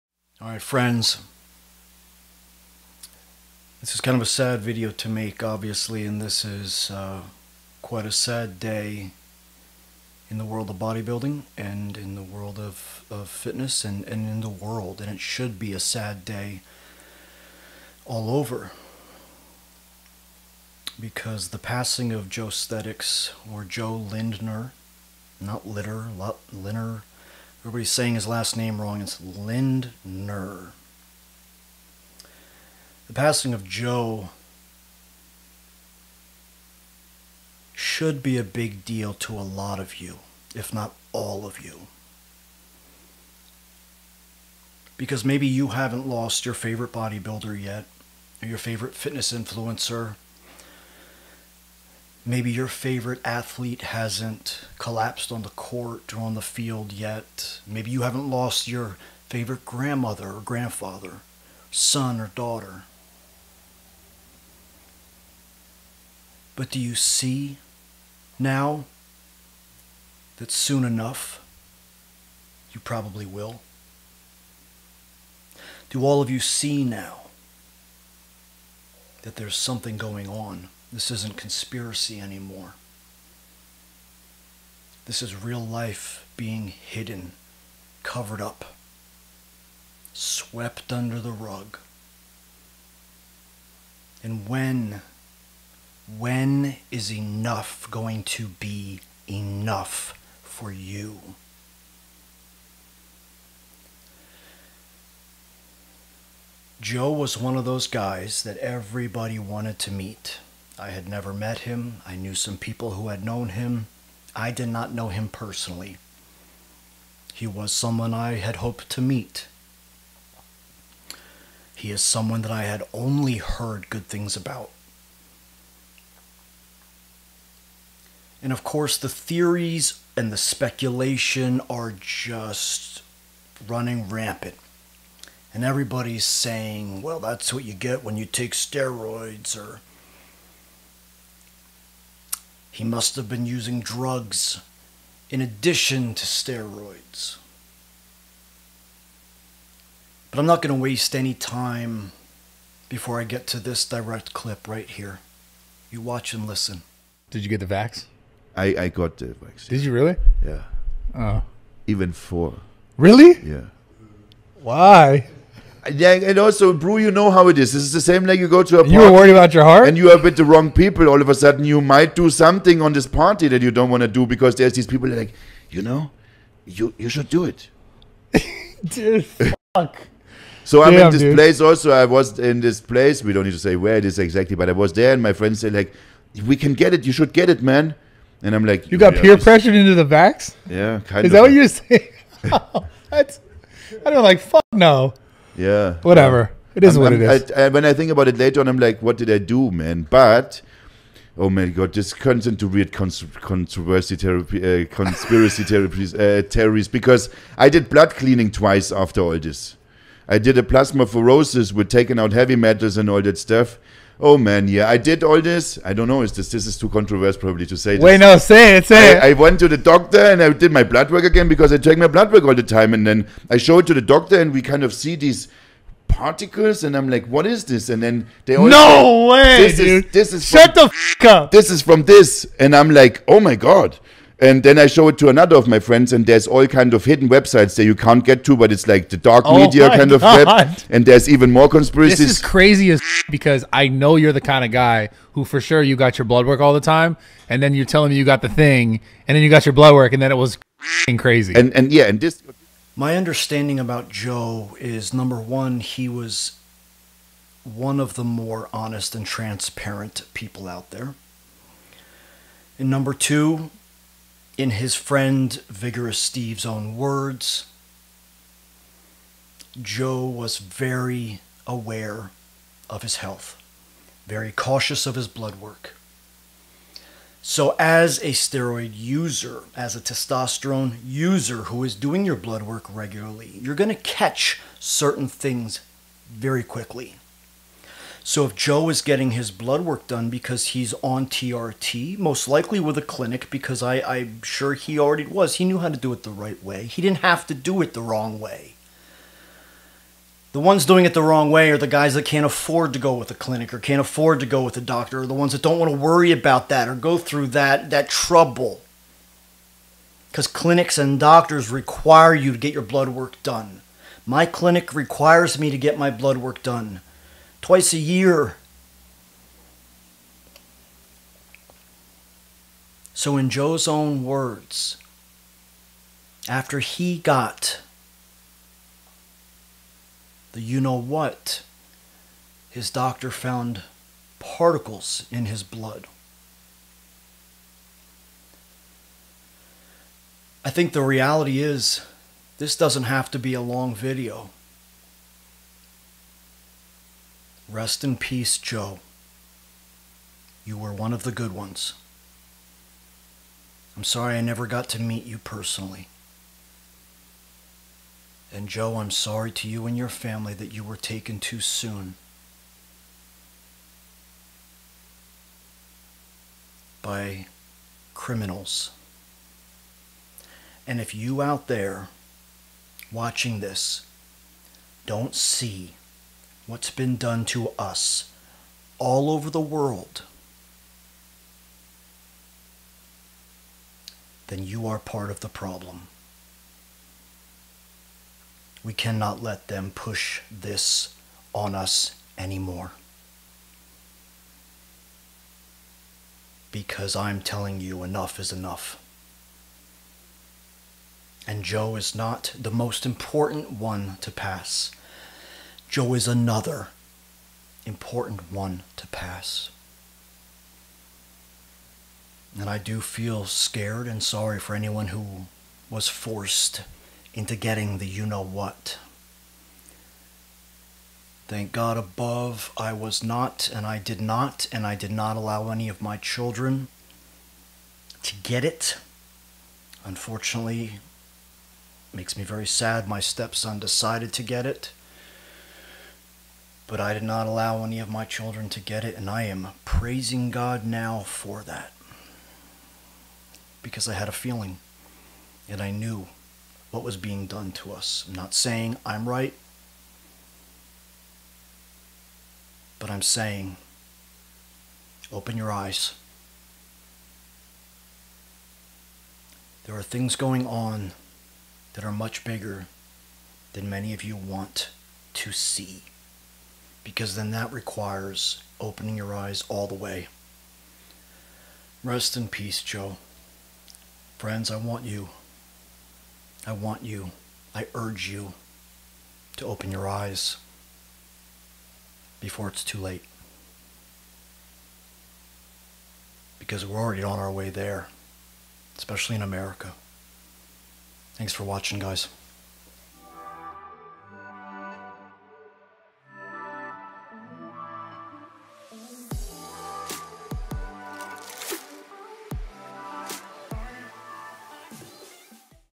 JoeStethics Lindner passes at 30 - the TRUTH affects us all!!
This video is about Joestethics